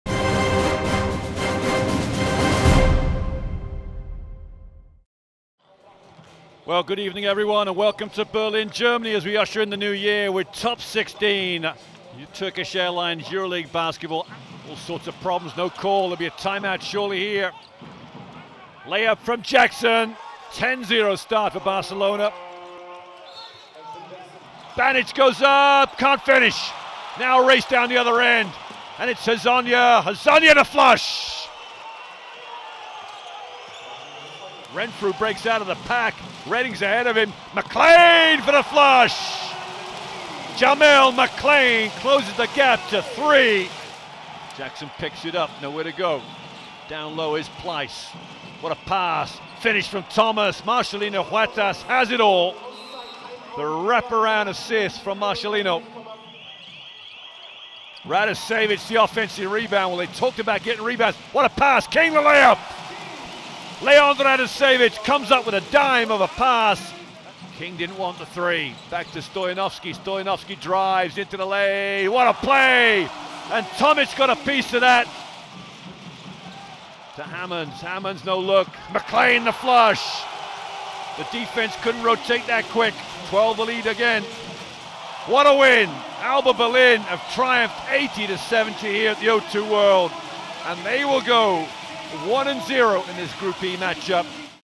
Highlights: ALBA Berlin-FC Barcelona
ALBA Berlin took charge in the middle two quarters to beat FC Barcelona 80-70 on Friday night in both teams’ Top 16 opener at o2 World in the German capital. Reggie Redding, Jamel McLean and Leon Radosevic scored 16 points apiece to pace the hosts.